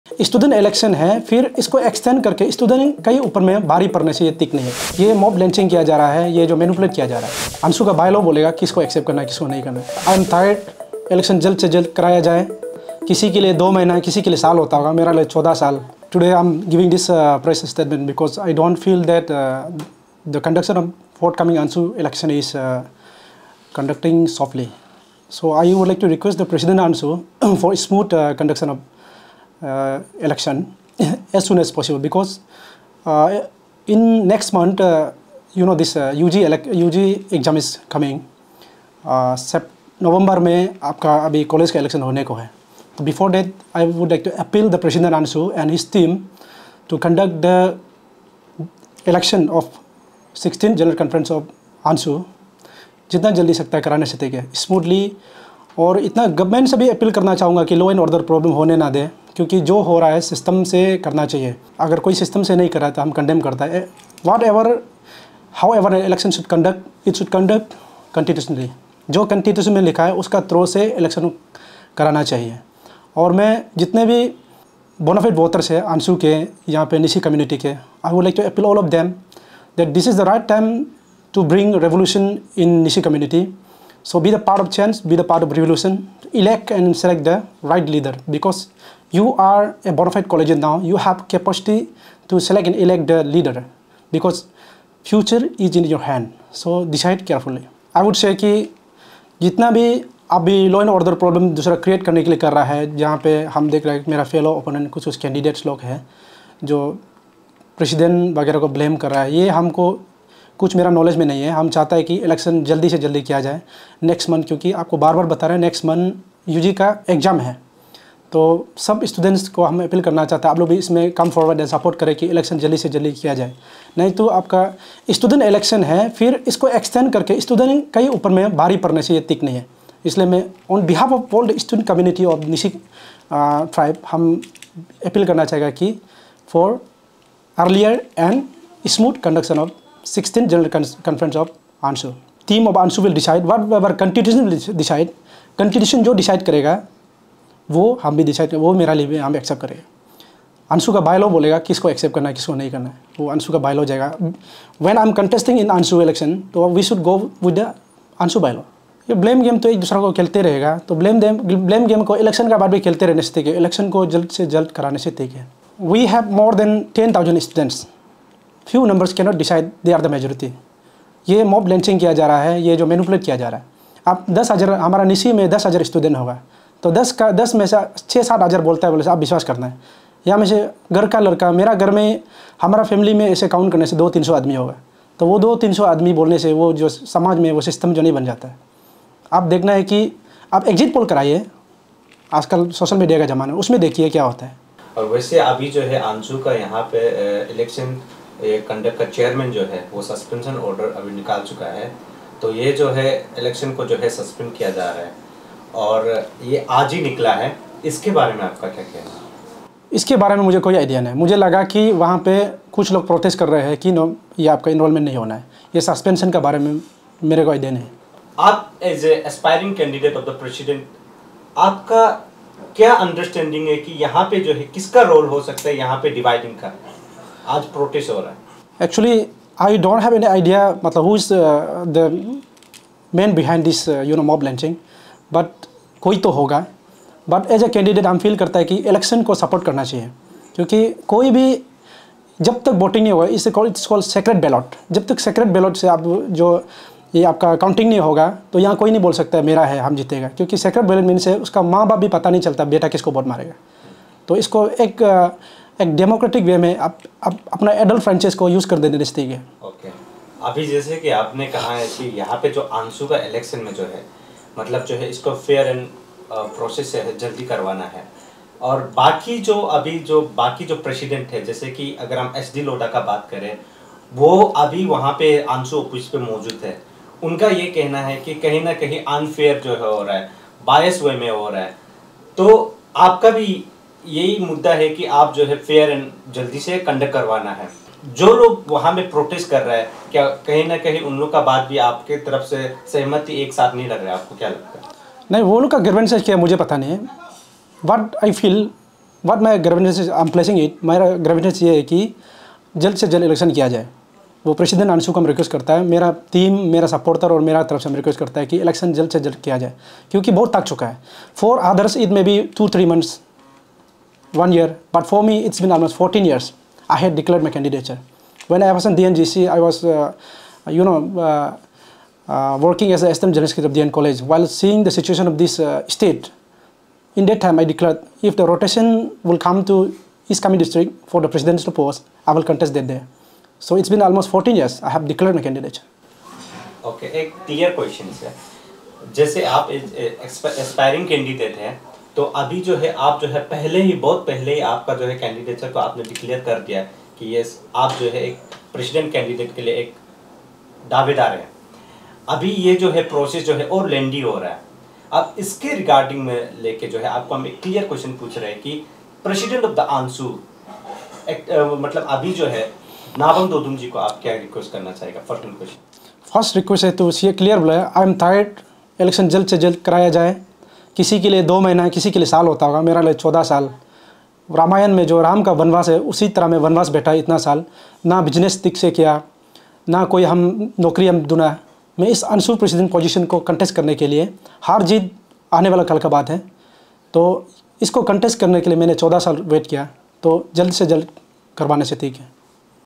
Lezen Gyadi Calls for Timely ANSU Election 2024 to Ensure Effective Governance.
Lezen Gyadi, an aspiring presidential candidate for the upcoming ANSU Election 2024, has emphasized the need for the elections to proceed as scheduled. His call highlights the importance of timely elections to ensure effective governance and representation within the organization.